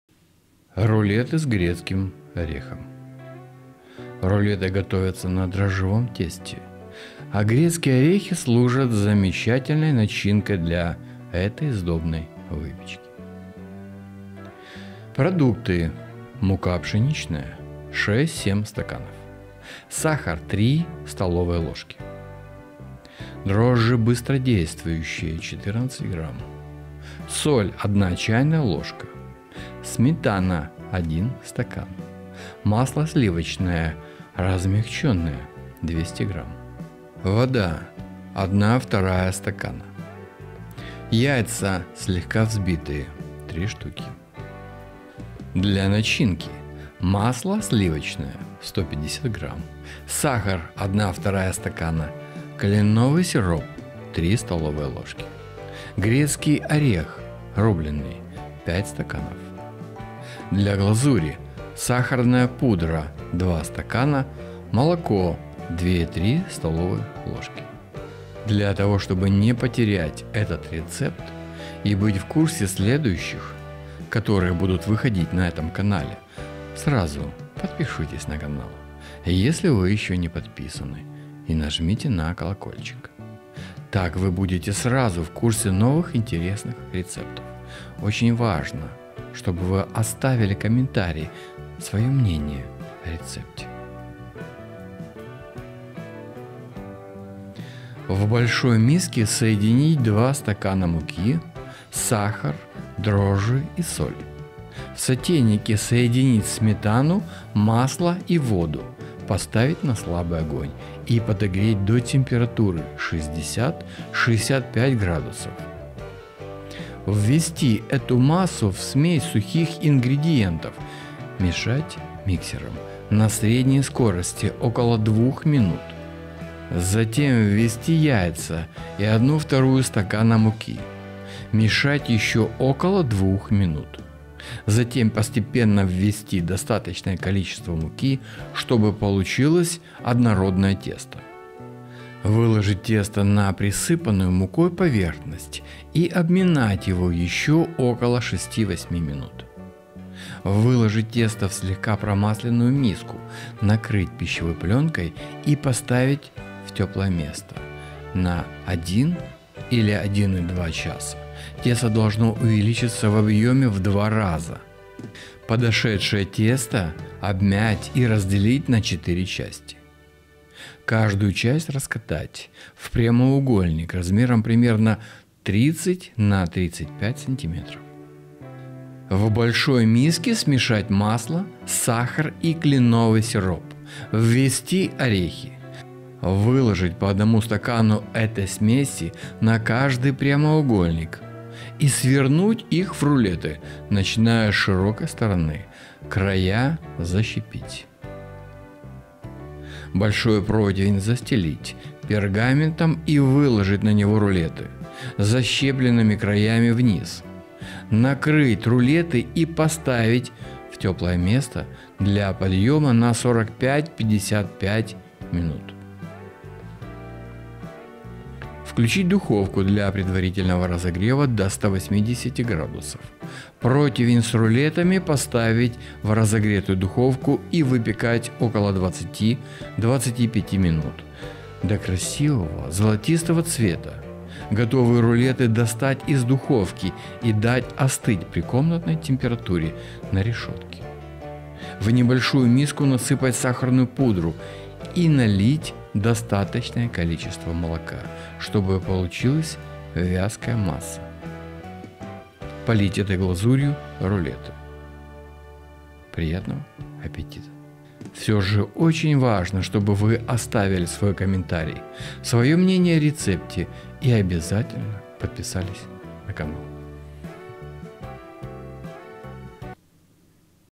Рулеты с грецким орехом
Продукты
Мука пшеничная - 6-7 стаканов
Сахар - 3 ст. л.
Дрожжи сухие быстродействующие - 14 г
Соль - 1 ч. л.
Сметана - 1 стакан
Масло сливочное (размягченное) - 200 г
Вода - 1/2 стакана
Яйца (слегка взбитые) - 3 шт.
Для начинки:
Кленовый сироп - 3 ст. л.
Грецкий орех (рубленый) - 5 стаканов
Для глазури:
Сахарная пудра - 2 стакана
Молоко - 2-3 ст. л.